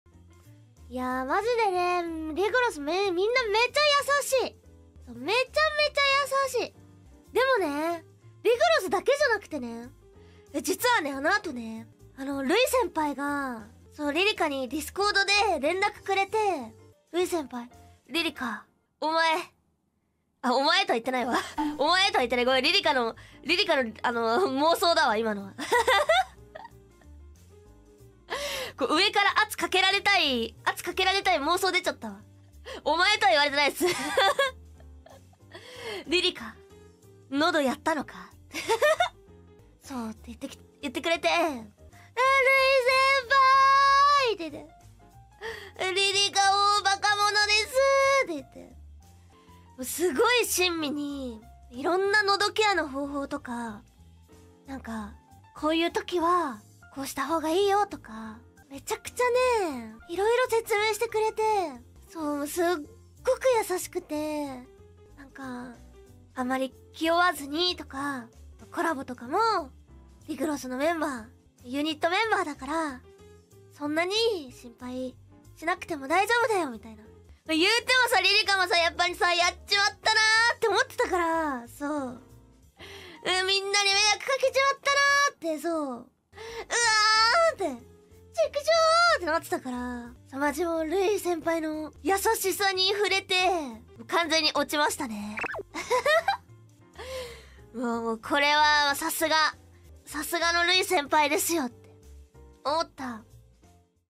Ririka fell for Lui's kindness after getting advices from her 【 ENG Subs 】
Source : 【雑談】一緒にお昼食べながら状況報告！！！今週のスケジュール調整します！！心配かけてごめんなさい！！！【一条莉々華/hololive DEV_IS ReGLOSS】

I mean, who wouldn't fall for Lui though. So glad to see them interacting this much.

If there's any mistranslation or error, please let me know!